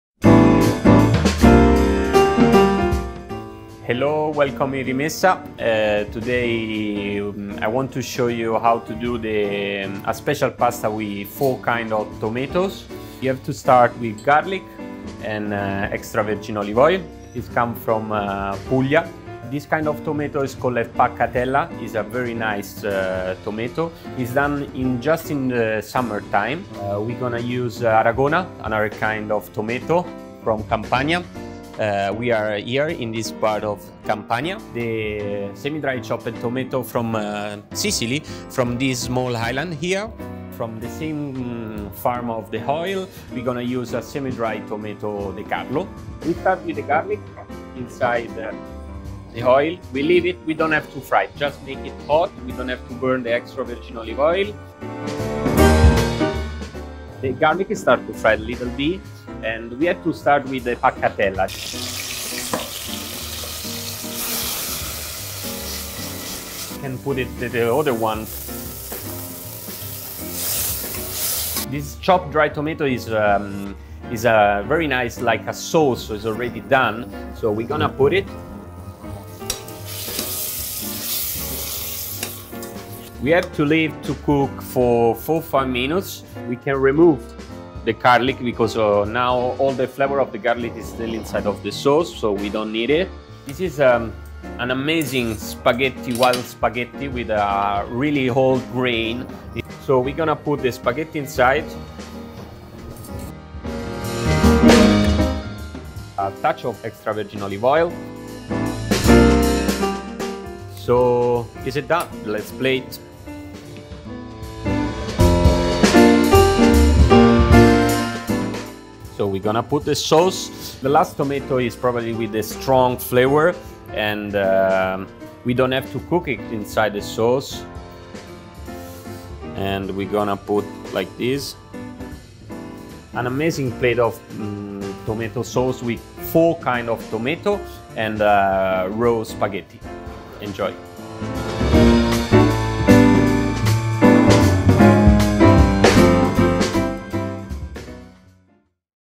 Rimessa Roscioli Ricetta 3 Pomodori
Chef Tommaso shows us how to make 3 Pomodori at Rimessa Roscioli.  Prepared during our lunch tasting demonstration and cooking class, when in Rome!

FOLLOW US / SEGUICI